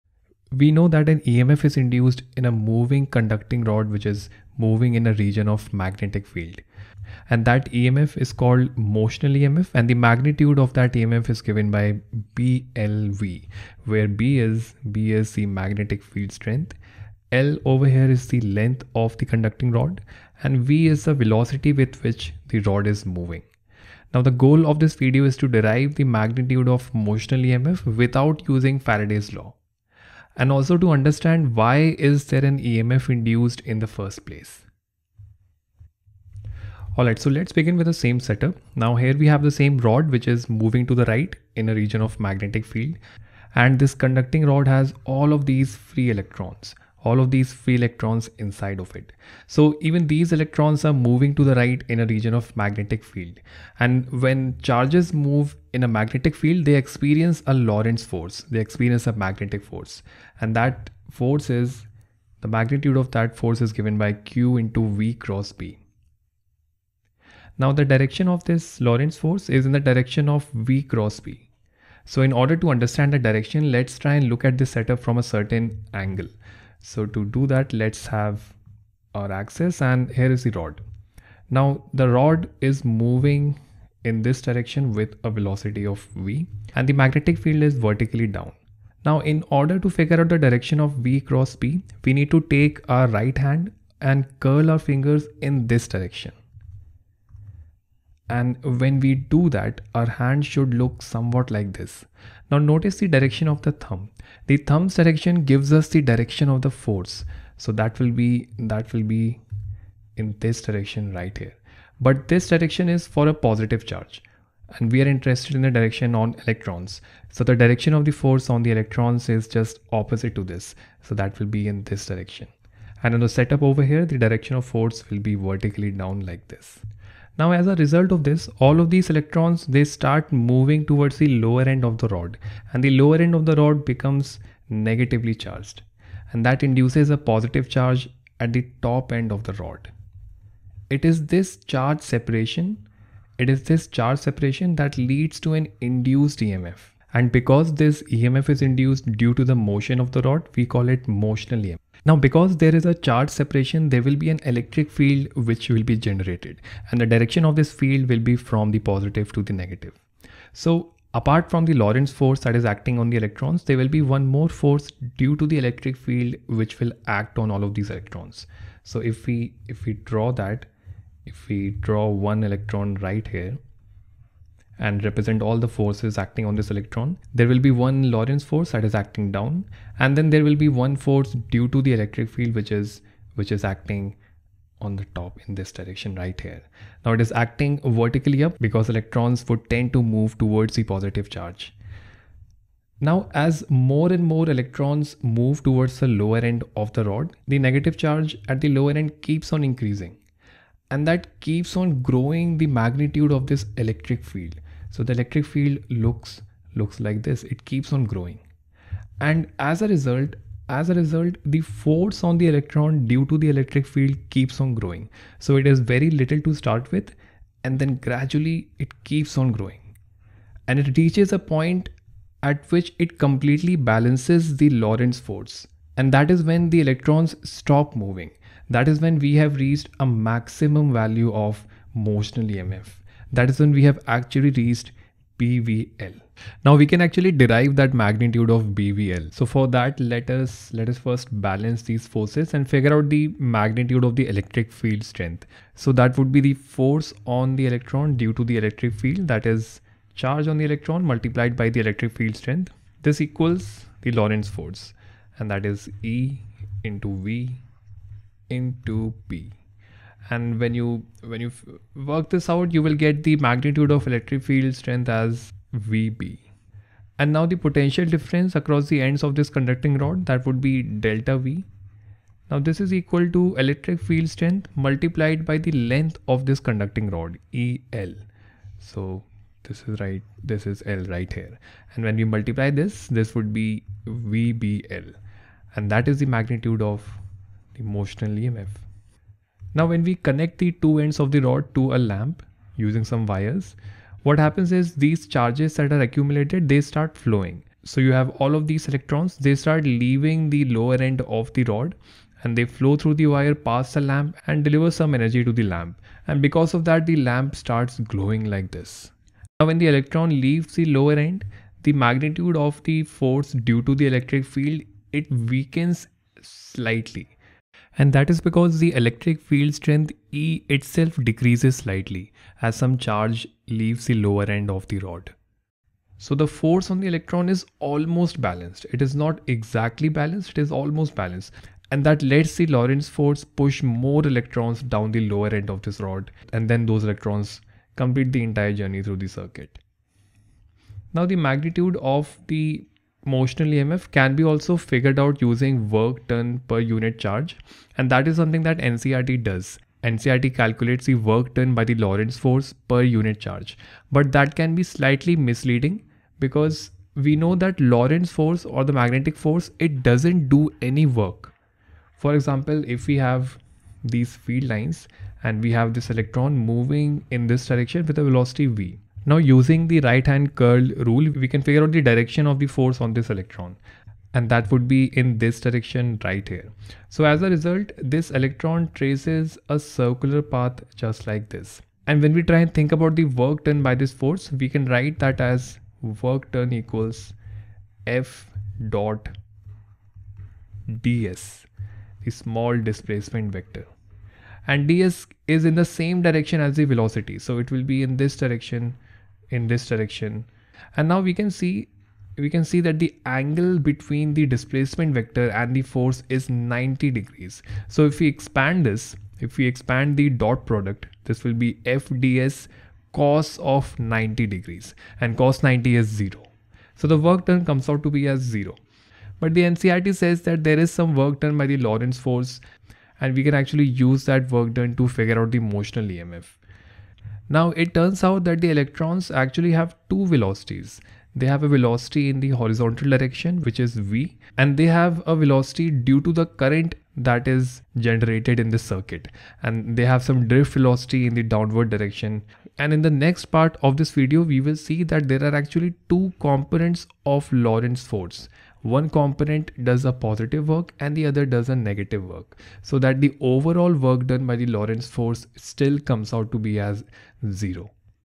Part 1 - Why is an EMF induced in a moving rod? | EMI | Physics | Khan Academy
In this video, we explore why is an EMF induced in a moving conducting rod and derive the magnitude of motional EMF without using Faraday's Law

More free lessons & practice "Link"